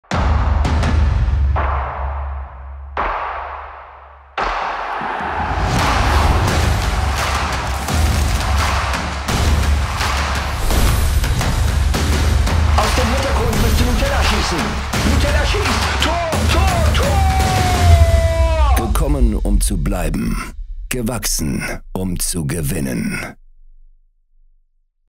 FERRERO EIS // WERBECLIP
Sommer, Sonne, EM und dazu das neue Nutella-Eis von Ferrero: In Zusammenarbeit mit der Agentur Kamikaze durften wir einen Produktfilm konzipieren und umgesetzen, der den Führungskräften von Ferrero Ende 2023 bei deren interner Versammlung vorgeführt wurde, um im EM-Jahr 2024 mit den neuen Eissorten für Begeisterung zu sorgen. 🎉

Unser Animationsclip zeigt Euch nun kompakt, wie wir die gesamte "Mannschaft" der neuen Eissorten kreativ präsentieren - mit dem Nutella-Eis als neuem Star im Sturm! Das Ergebnis beweist, dass wir neben dem Drehen auch Animationen beherrschen, um die Produkte renommierter Marken wie Ferrero visuell in Szene zu setzen! 🎥